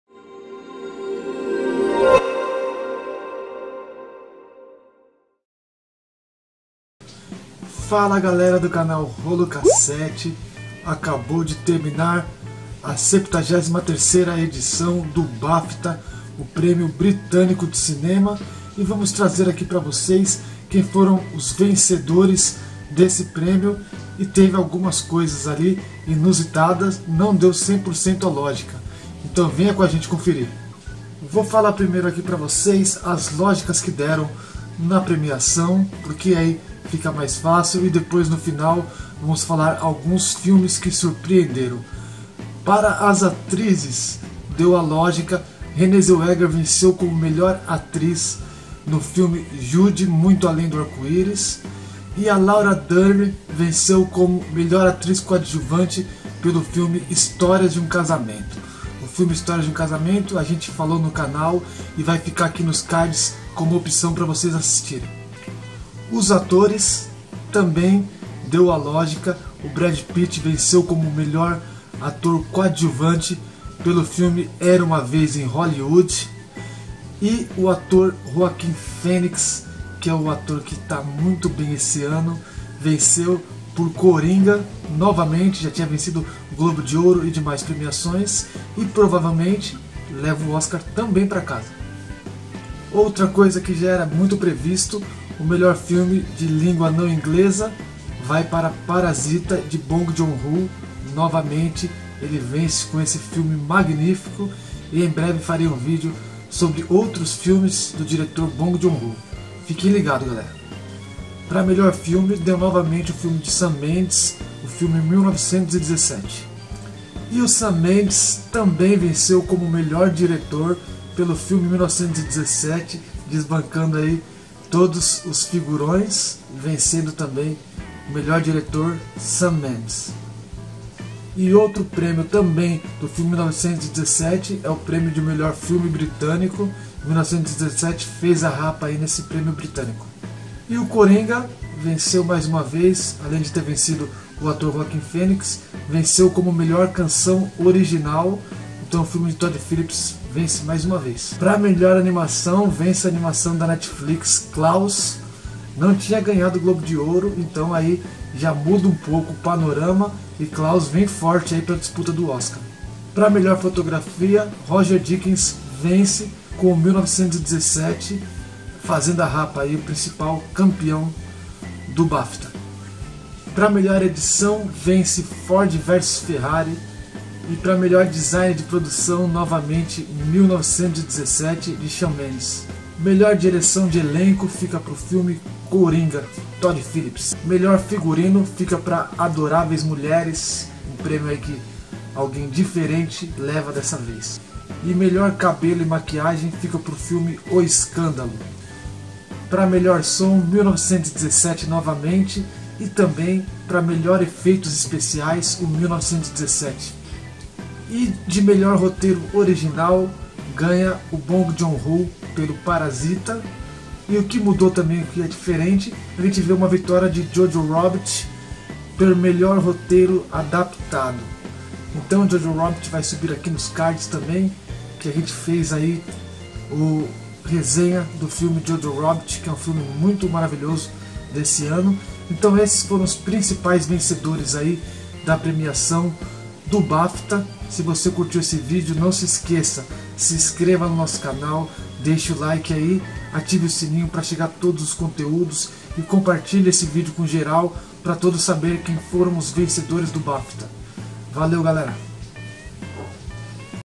Bafta 2020 - Conheça os vencedores
Conheça quem foram os vencedores do BAFTA 2020, as logicas e as novidades esse é mais um termômetro para vermos quem serão os vencedores do Oscar 2020.

TWEETER: @k7rolo